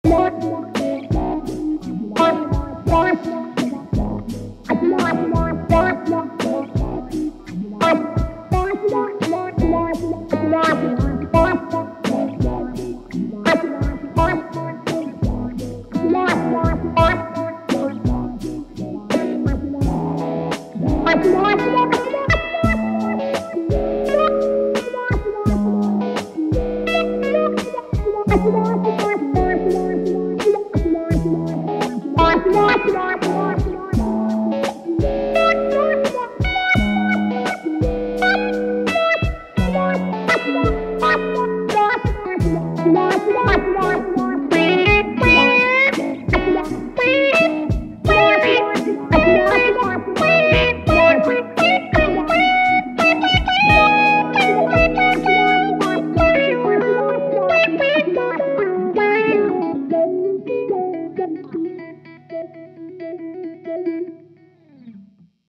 GFI System Rossie Filter // Sonic Snack
GFI system knows what they're doing. Coming off of the Orca Delay and Skylar Reverb, the Rossie Filter comes in the similar form and function of it's recent predecessors. This extracts the Envelope Filter from it's big brother, the GFI System Synesthesia, and adds more tweakability and a few new modes - Step Filter and Manual Filter. This small package is packed with versatility: presets, tap, and a screen to help you navigate the parameters, secondary functions, and global settings. Filer on my friends!
Signal Chain:
Fender '72 Thinline Telecaster
Lambertone Cremas
920D Custom Wiring
Benson Preamp
GFI System Rossie Filter
Strymon Flint and Iridium